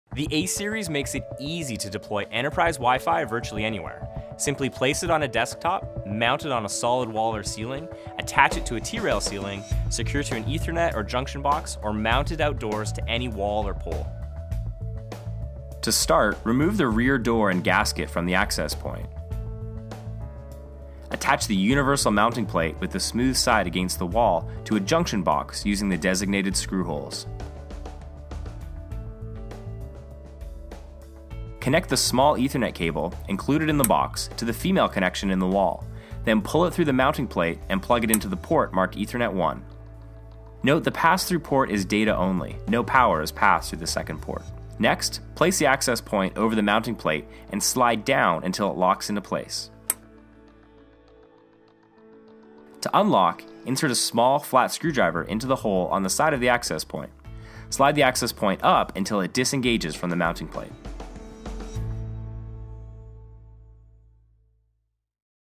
A Series Installation: Ethernet or Junction Box
In this video Ryan from Open Mesh shows you how to secure the A Series to an Ethernet or junction box.

The A Series makes it easy to deploy enterprise WiFi virtually anywhere. Simply place it on a desktop, mount it on a solid wall or ceiling, attach it to a T-rail ceiling, secure to an Ethernet or junction box, or mount it outdoors to any wall or pole.